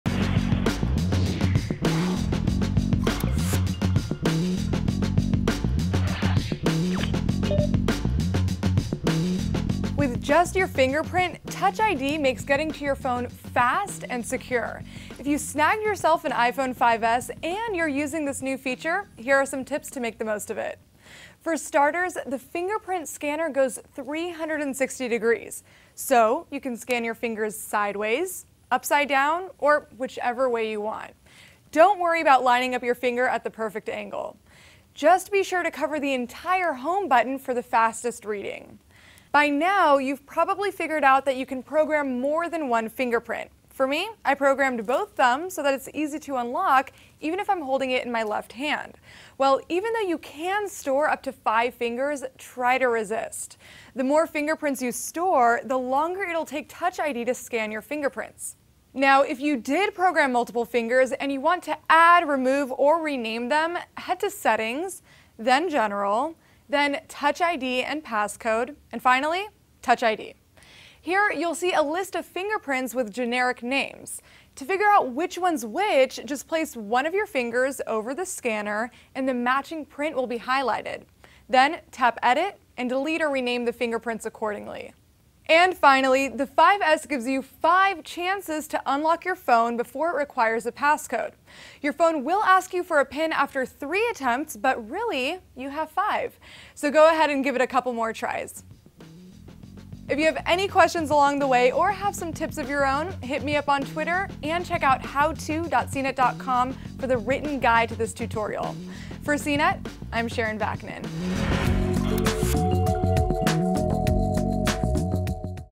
CNET How To - Tips for the iPhone 5S' fingerprint scanner
Get more out of Touch ID with these little-known tips and tricks.